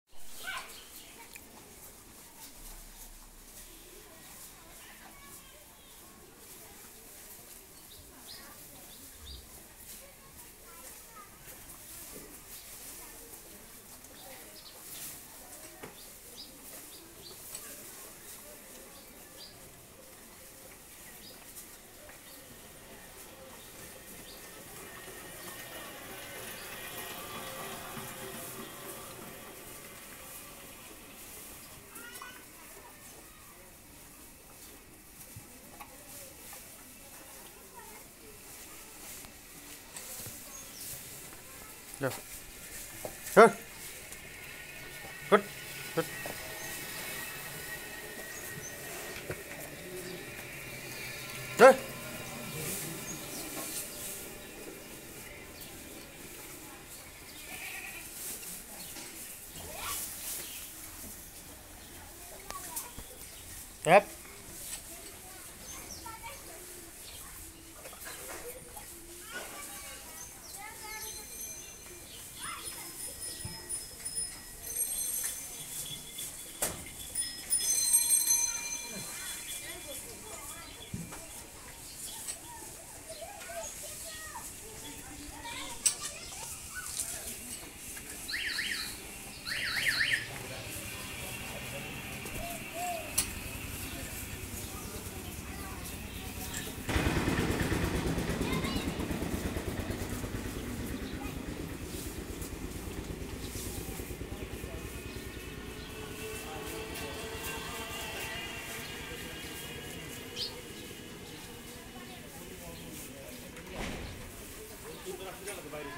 ৩২৪১ নং ফ্রিজিয়ান জাতের গাভীন বিক্রয় হইবে।01742082521//01735966692
এটি একটি উন্নতমানের ফ্রিজিয়ান জাতের গাভীন।গাভীটি এবার ১ম বারের মত বাচ্চা প্রসব করবে।পেটে ৯ মাসের বাচ্চা আছে।